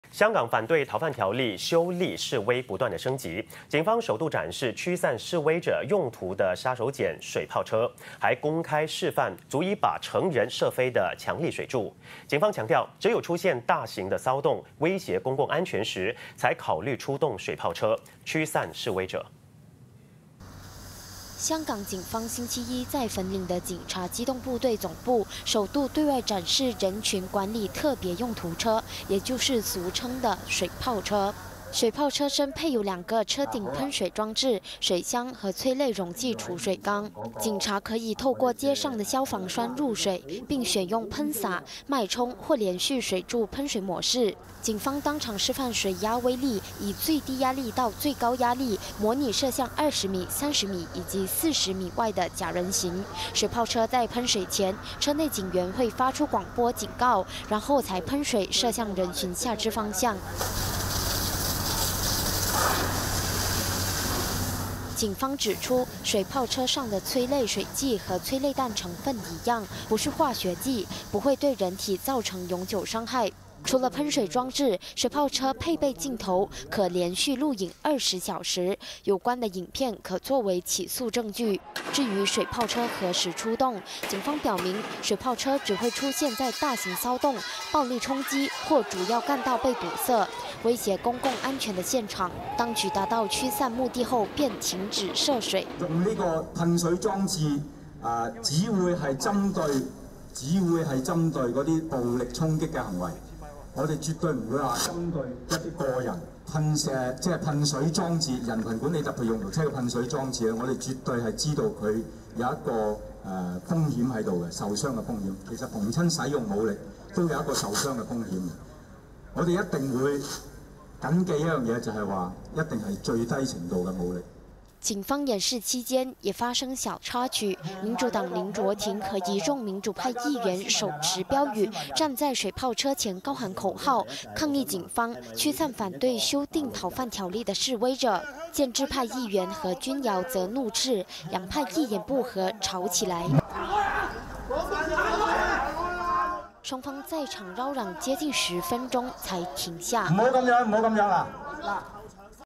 港警展示水炮车火力 强力水柱人可射飞
香港反对逃犯条例修例示威不断升级，警方首度展示驱散示威者用途的杀手锏-水炮车，还公开示范足以把成人射飞的强力水柱。警方强调，只有出现大型骚动威胁公共安全时，才考虑出动水炮车，驱散示威者。